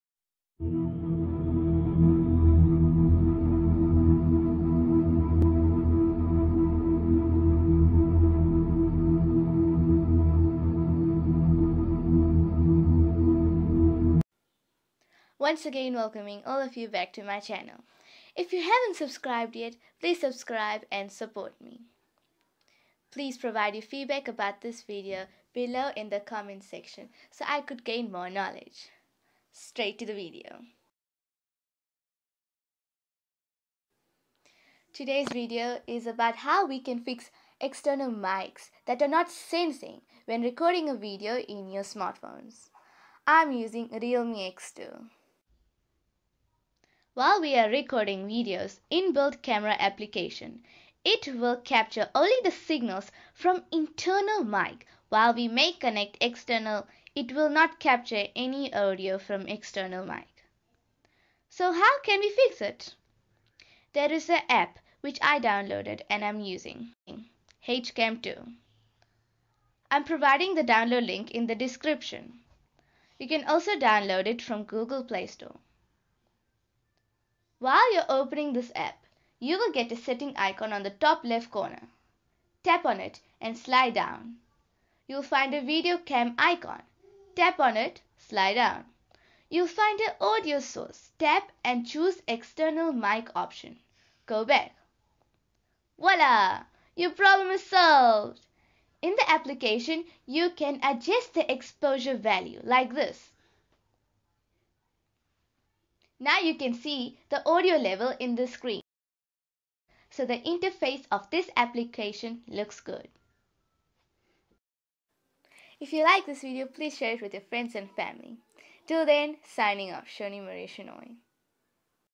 How we can fix external mic problems in your smartphone
Some smartphones didn't sense external mic when  recording with its in built camara application.

Here we can install an application named hedge cam 2 which you can choose the mic sources.
Download link

Or you can download directly from Google play store

Enjoy your application

Please comment your feedback